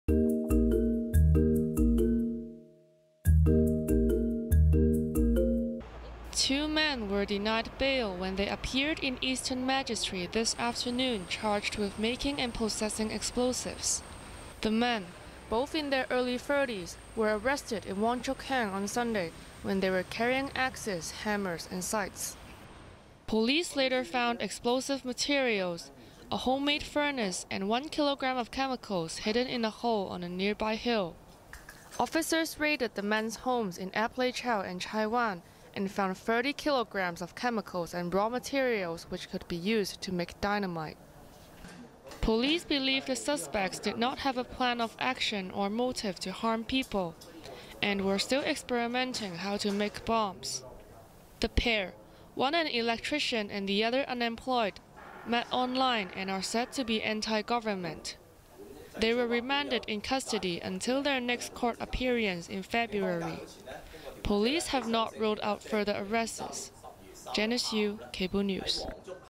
Two Denied Bail on Bomb-making Charge | HOY International Business Channel
Two men were denied bail when they appeared in Eastern Magistracy this afternoon charged with making and possessing explosives.

The men, both in their early thirties, were arrested in Wong Chuk Hang on Sunday when they were carrying axes, hammers and scythes.